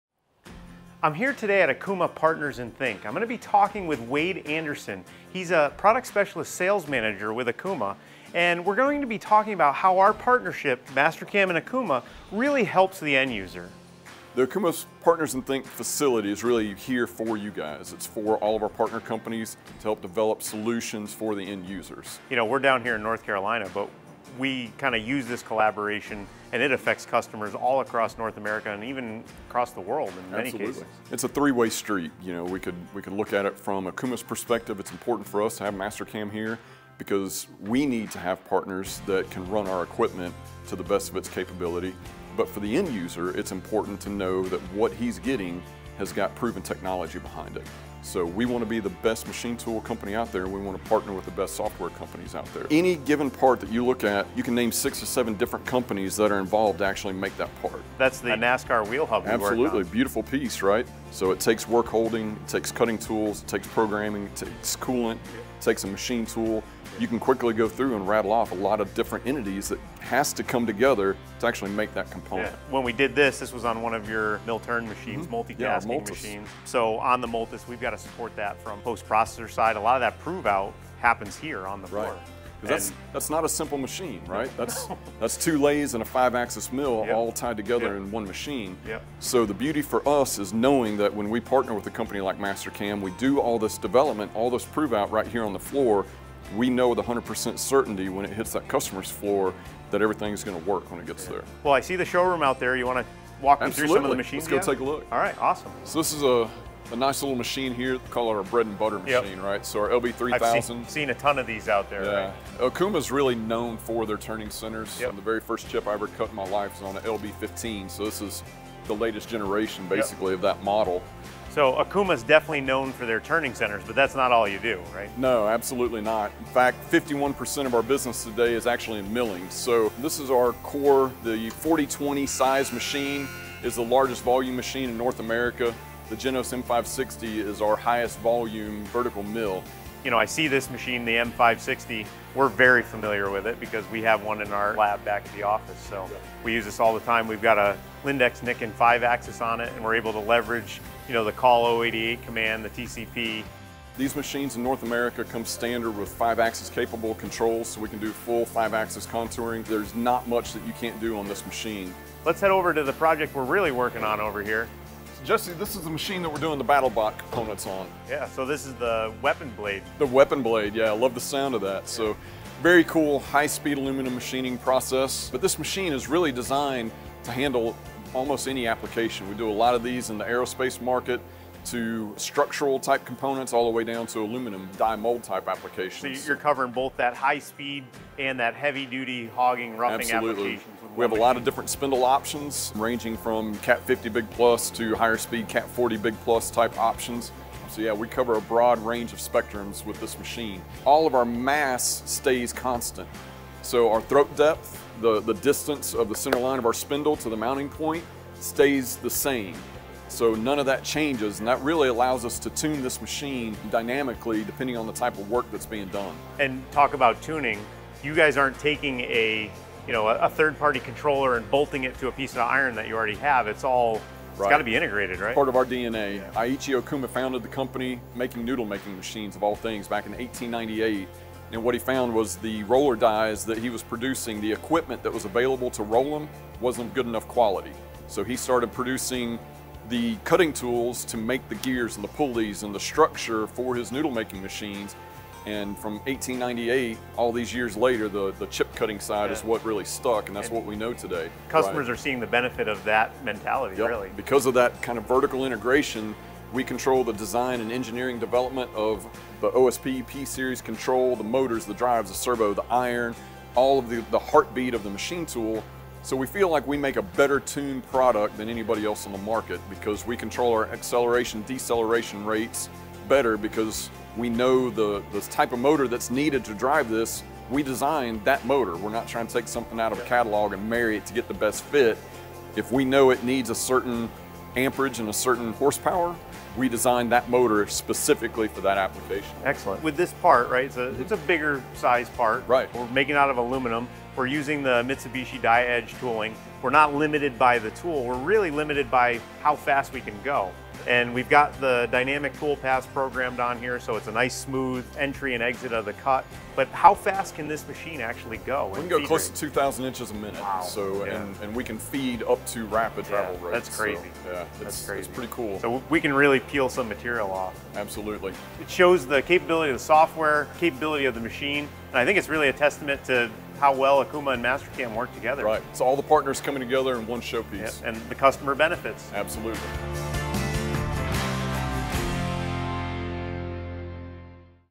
Okuma America | Partnership Spotlight
Jesse and @OkumaAmericaCorp Product Specialist Sales Manager, Wade Anderson, discuss how the Okuma and Mastercam partnership allows the end user to better utilize their products.

From turning centers to vertical milling machines and much more, Okuma's vast lineup allows them to carry out some of the most difficult manufacturing jobs with ease.

Join the Mastercam community on social